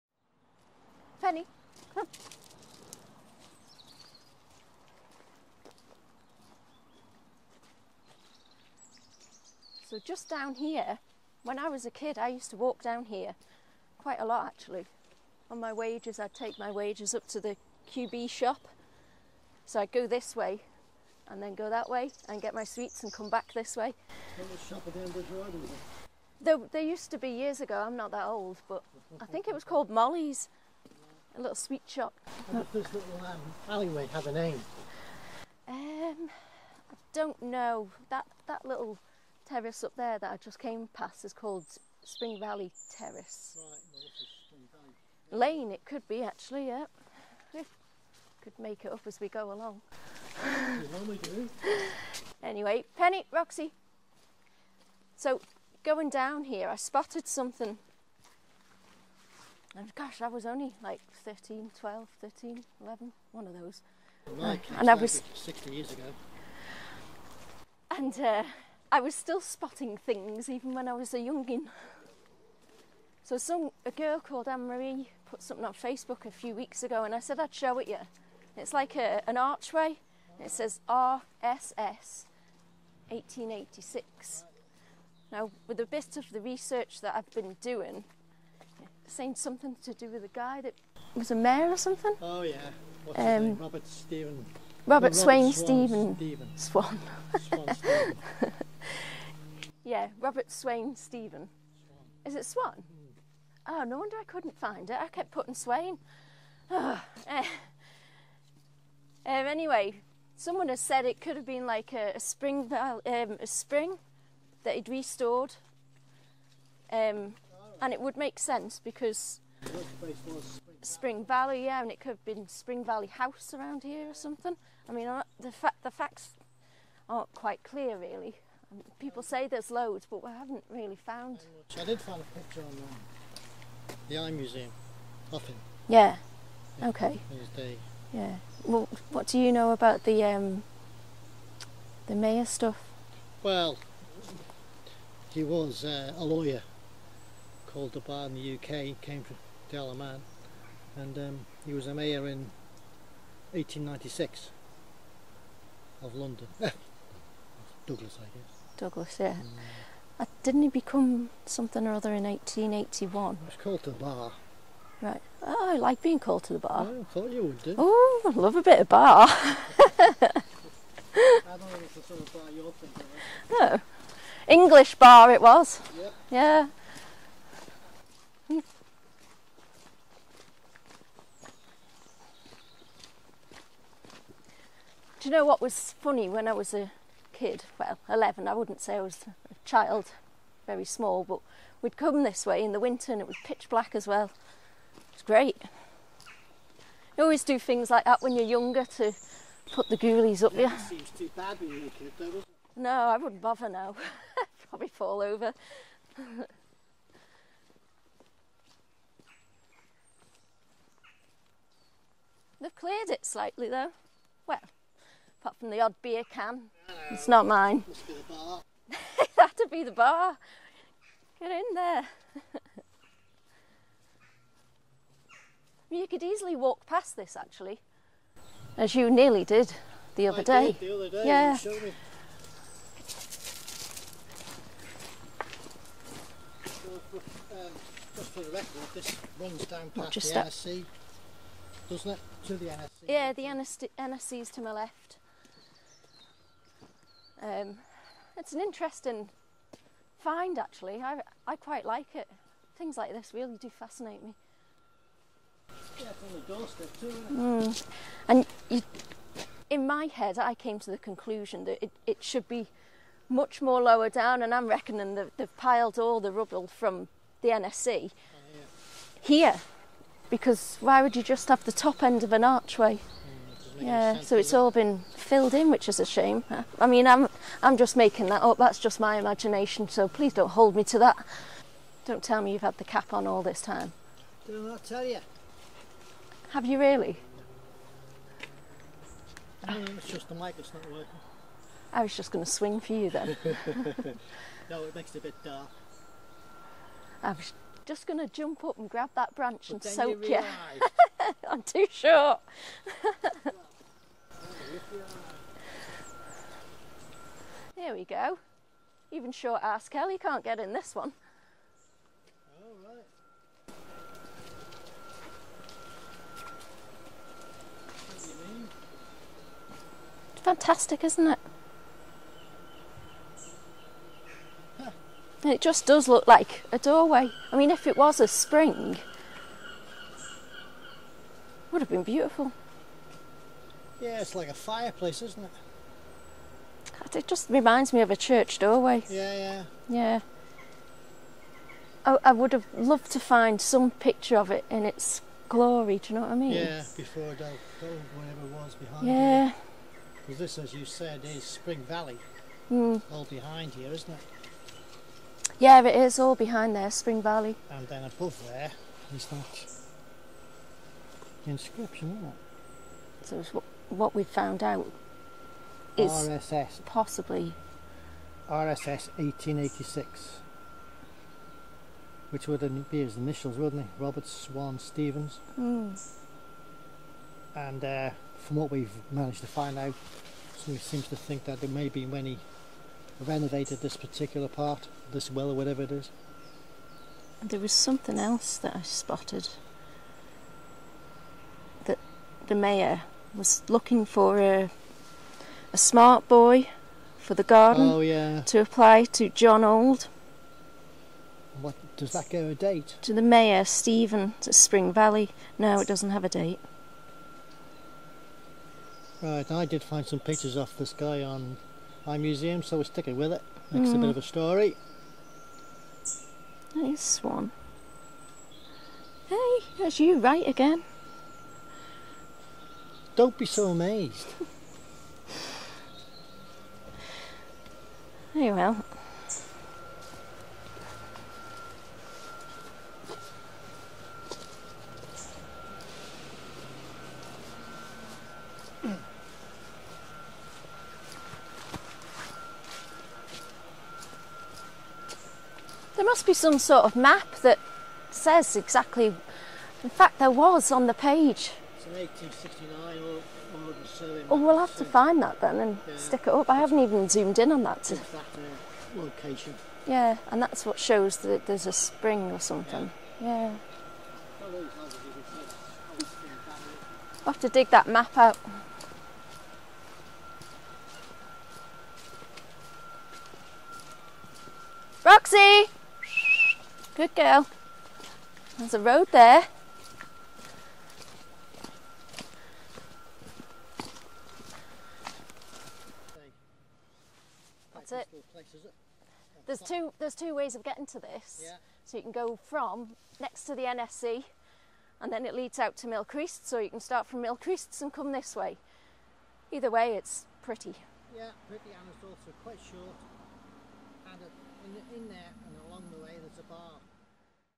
Ray & Carla, Isle Of Man, Douglas, Robert Swan Stephens Plague
On a little footpath in Douglas town, trod by many, and noticed by a few, is a little opening into another world, conjecture has given it many answers, but the mystery still remains, was it a well, or a fairy door, was it the entrance to a long since forgotten chapel, like all curiosities the people that know are now too history.
But walk with us while we wend our way along its meandering path, so close to the town, yet still quite secretive.

Ray & Carla